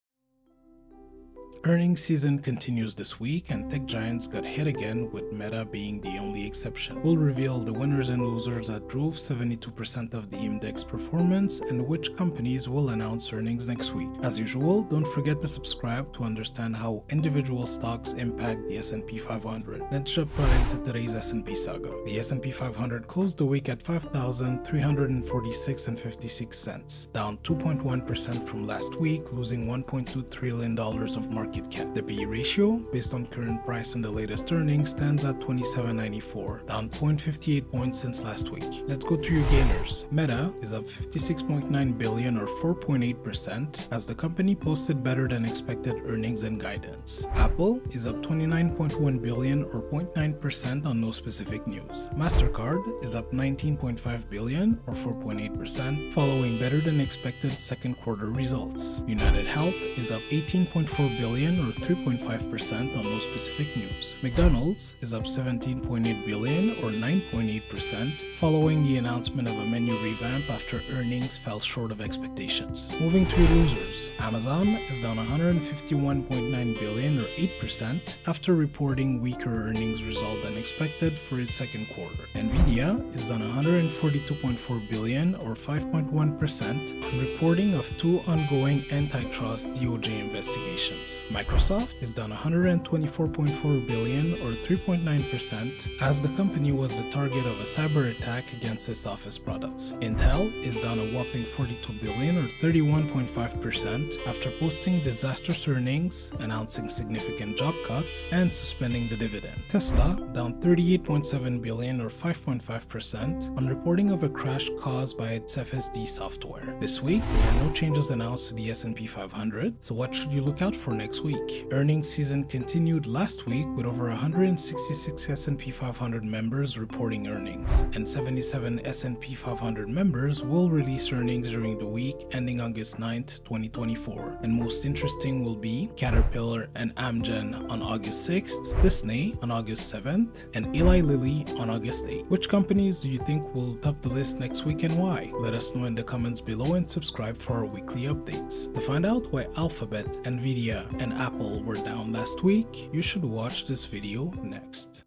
Amazon, Nvidia, Intel CRASH discussion + Q2 Earnings Finale
This week's movers: Meta, Apple, Mastercard, UnitedHealth Group, McDonald's, Amazon, Nvidia, Microsoft, Intel, Tesla, Caterpillar, Amgen, Disney, Lilly
Movers Symbols: META AAPL MA UNH MCD AMZN NVDA MSFT INTC TSLA CAT AMGN DIS LLY

Chapters
0:00 - Intro
0:22 - Last Week Market Performance
0:39 - Winners
1:13 - Losers
1:56 - Index Changes
2:07 - Earnings watch

August 6: Caterpillar and Amgen
August 7: Walt Disney Company
August 8: Lilly